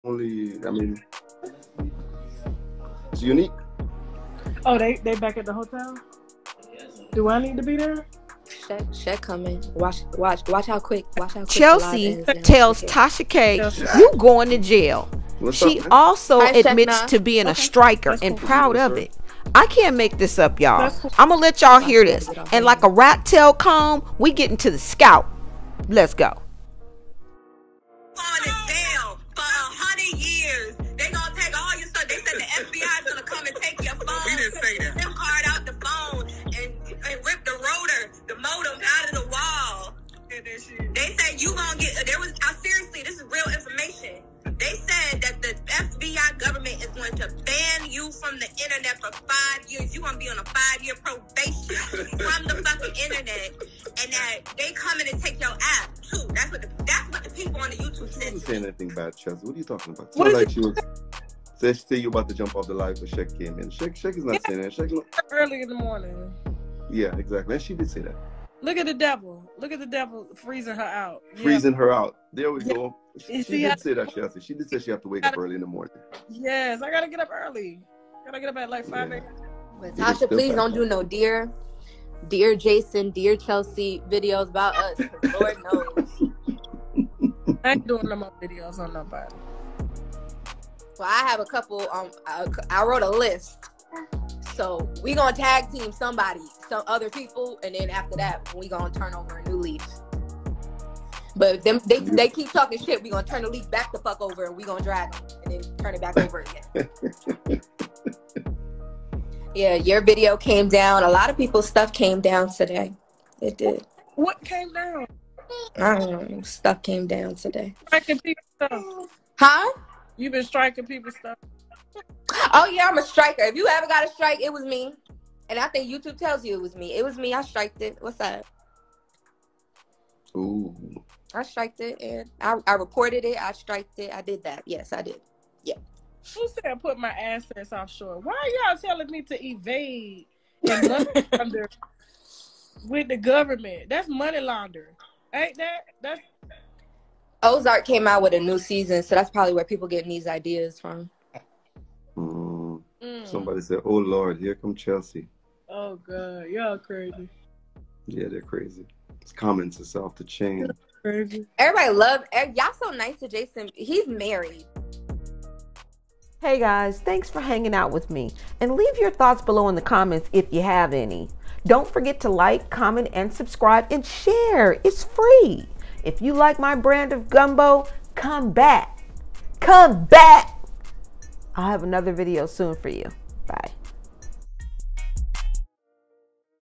Tasha K Going To Jail ? Chelsea Warns YT She’s A Striker ??
Tasha K Is going to jail! At least that’s what Chelsea says. Tasha K took to her Instagram live last night and boy did she spill some tea! Not only do we know exactly what Chelsea thinks…but we also know that she’s a striker. I’m just here to share the audacity.